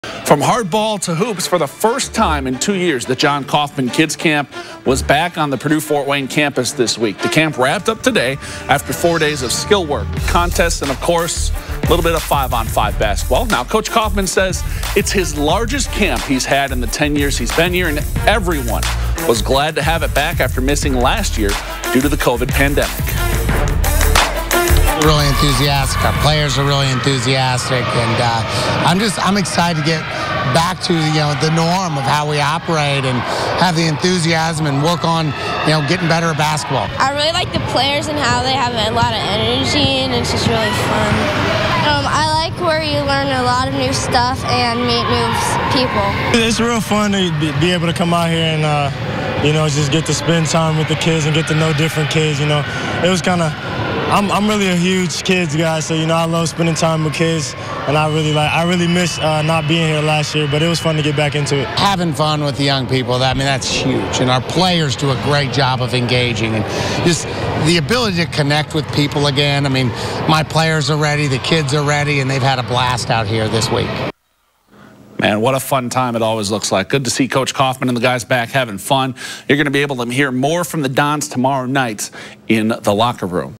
Annual Jon Coffman Kids Camp wraps up
The annual Jon Coffman Kids Basketball Camp has officially wrapped up following a week filled with fun, learning and, of course, basketball.